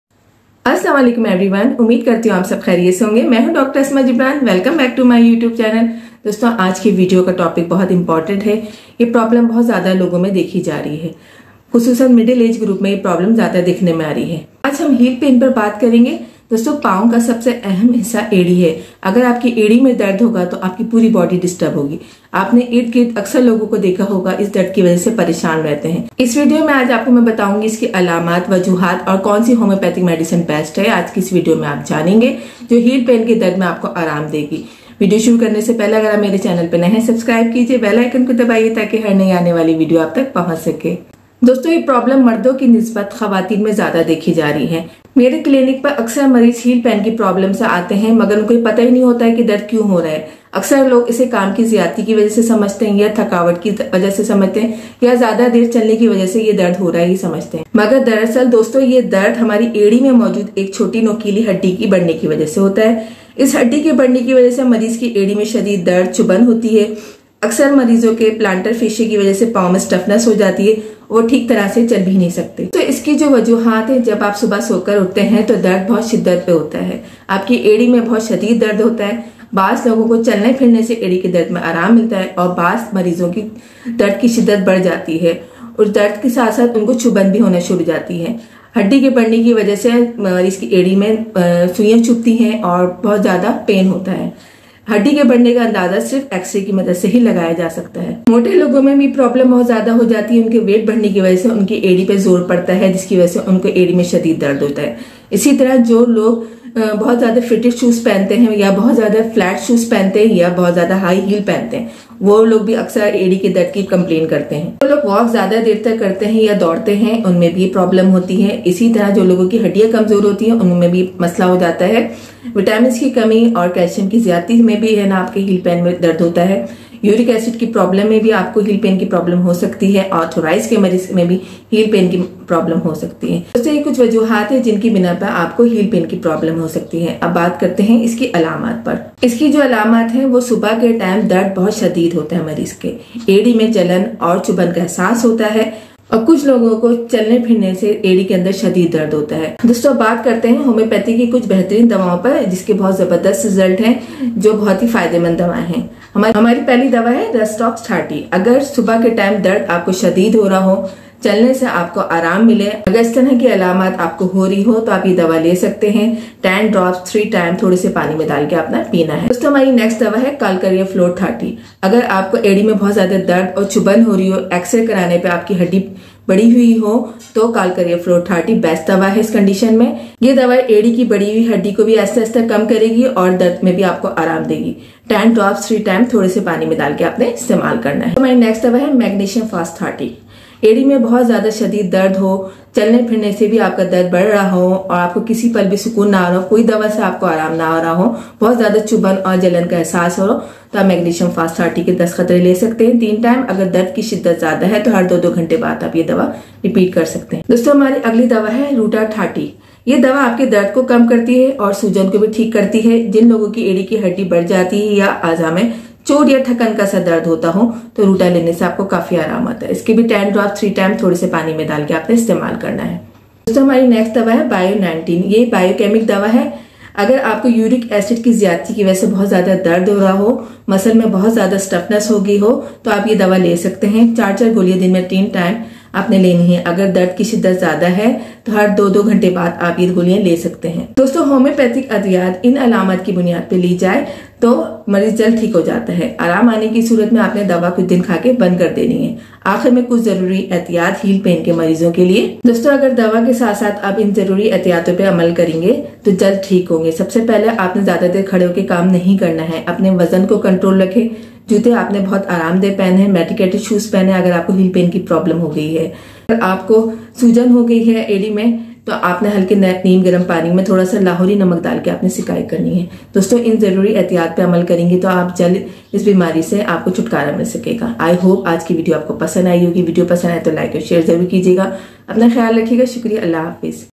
Heel Pain Relief || Causes & Treatment || ایڑی کے درد کا مکمل علاج || in urdu By Dr Asma Jibran
Hello Friends !!
I am Dr Asma Jibran watch this Video to know Heel pain causes symptoms and homeopathic medicines and treatment. I have explained what is Heel pain and very effective homeopathic medicines .
Thanks & Regards
        Dr Asma Jibran